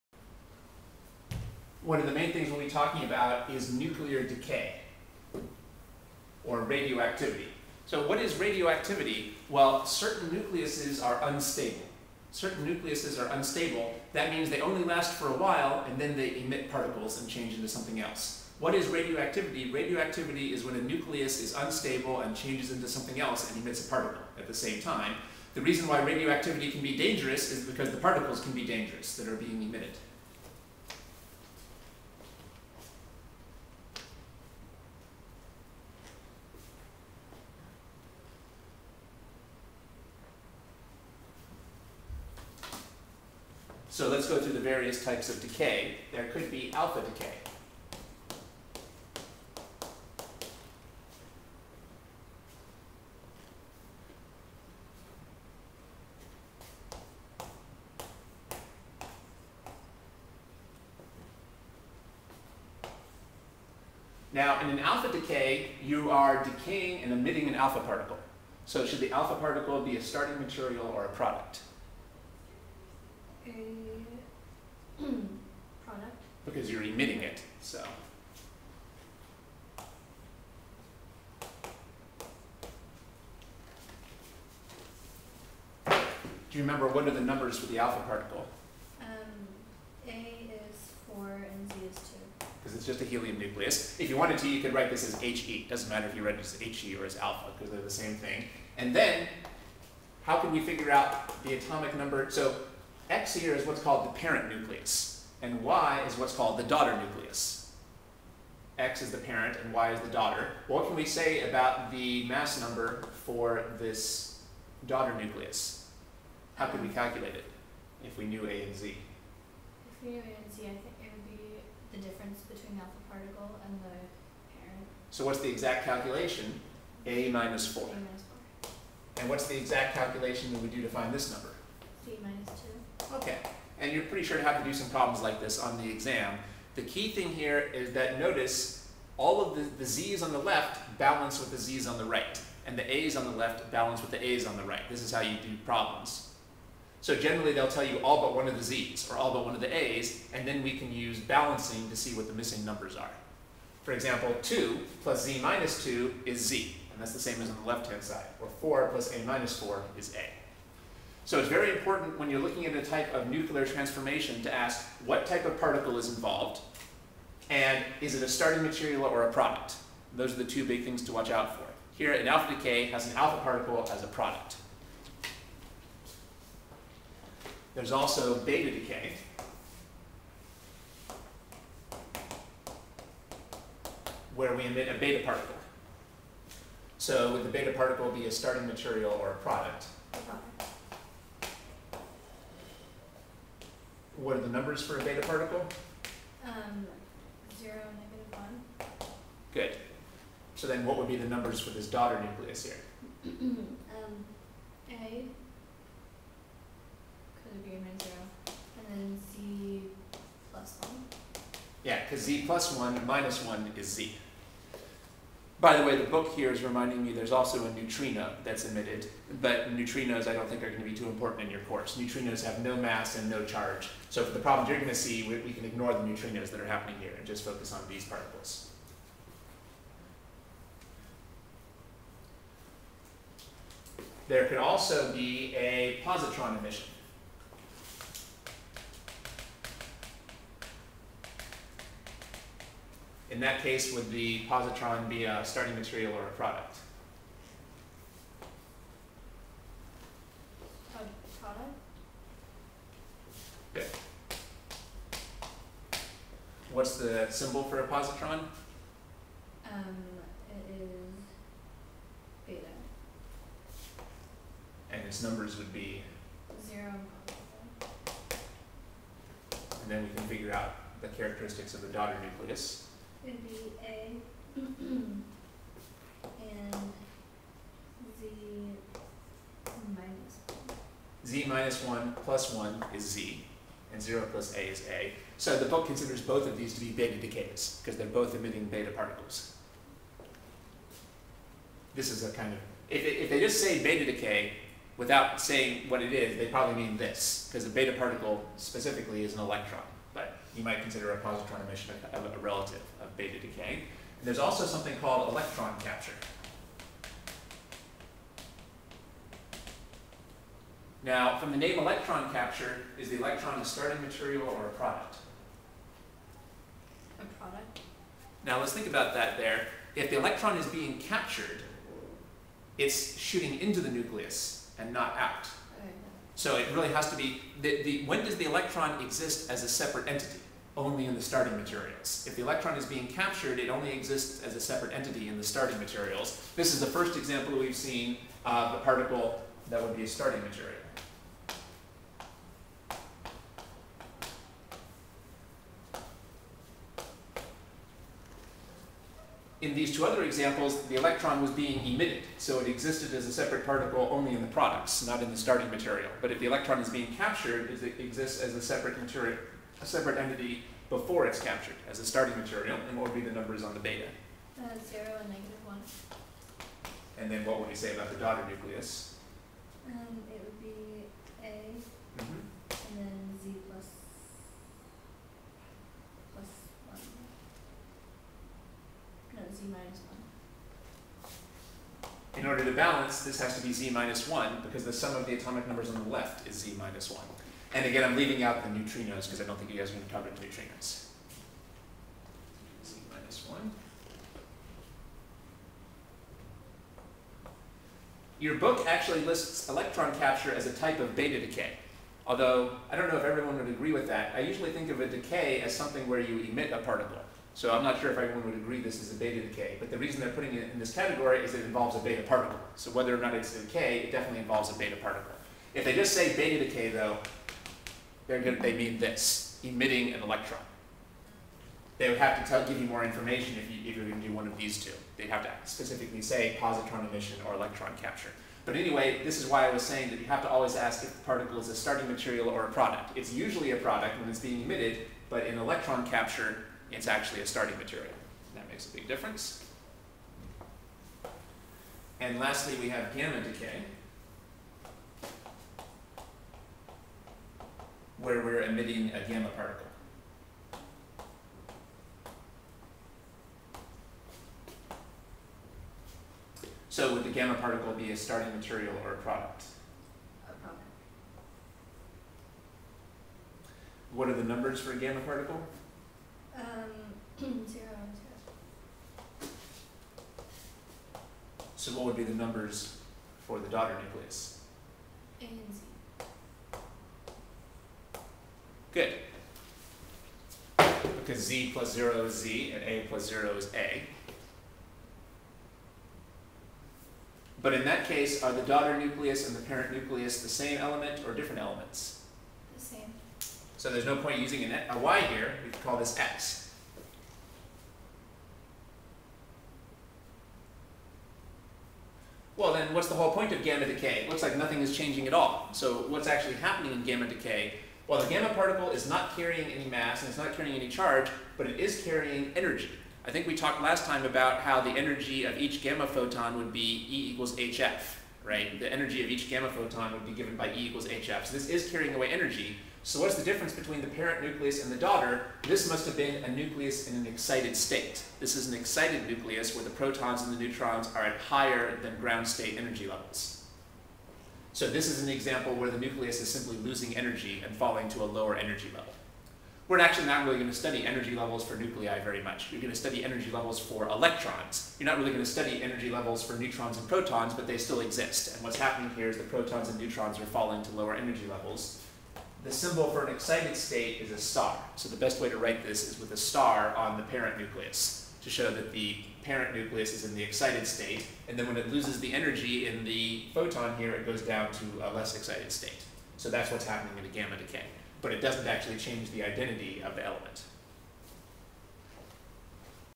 Nuclear physics (4)
Physics: Nuclear physics. Protons, neutrons, and electrons. Mass number (A) and atomic number (charge number, Z); conventional symbolism for nuclei. Alpha, beta, and gamma particles; alpha, beta, and gamma decay. Mathematics of radioactive decay; decay constant; half-life. Radioactive dating (carbon-14)

For a printable document containing the problems discussed in this video series, go to my website.

(1) Proton, neutrons, and electrons—mass and charge. Atomic mass units and elementary charge units
(2) Mass number (A), atomic number (charge number, Z)
(3) Conventional symbolism for nucleii. Alpha, beta, and gamma particles
(4) Alpha, beta, and gamma decay
(5) Problems
(6) Mathematics of radioactive decay; decay constant (lambda)
(7) A problem
(8) Half-life
(10) Continued. Relation between decay constant and half-life. A radioactive dating problem (carbon-14)